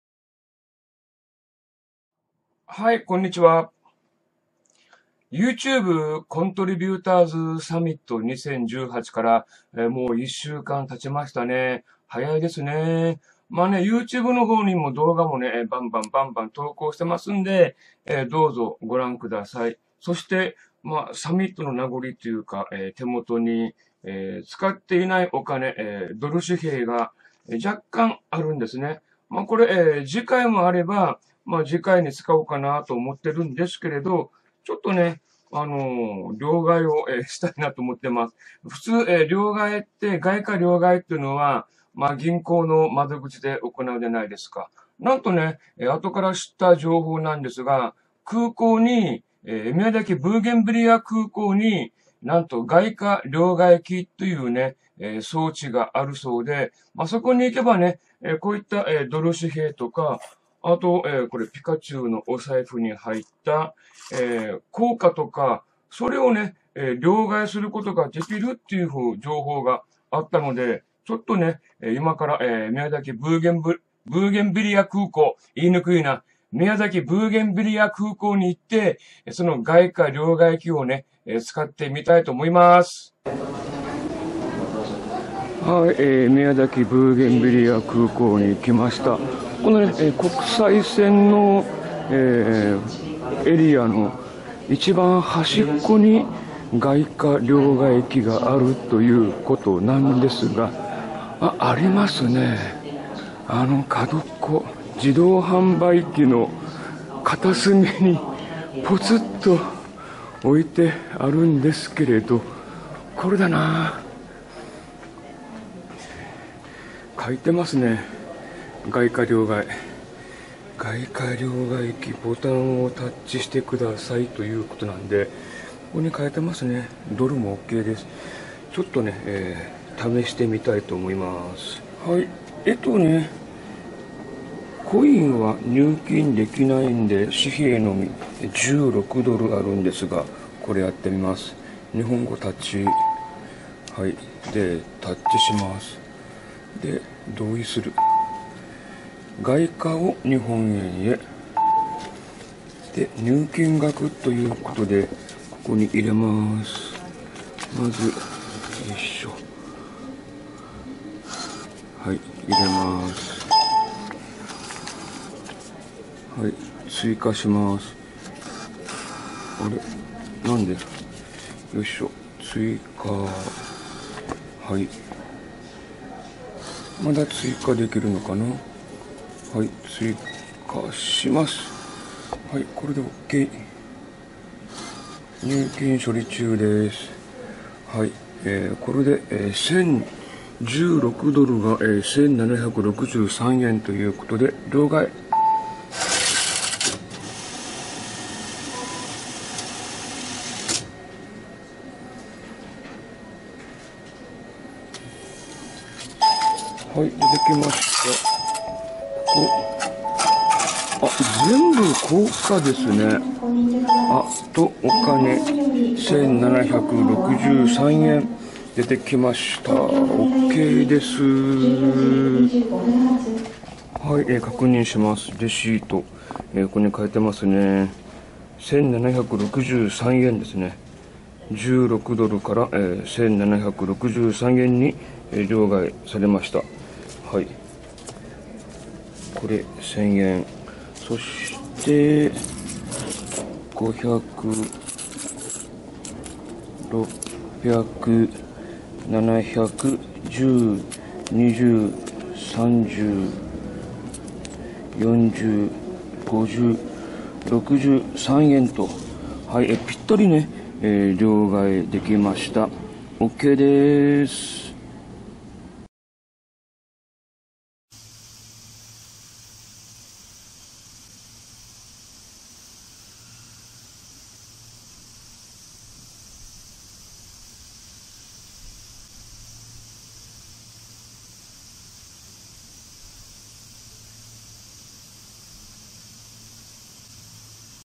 外貨両替機を使ってみた。
ドル紙幣と硬貨が少し残りました。通常であれば、銀行で両替するのですが、宮崎ブーゲンビリア空港に『外貨両替機』があるとのことで試してみました。なお、硬貨は両替できませんのでご注意ください。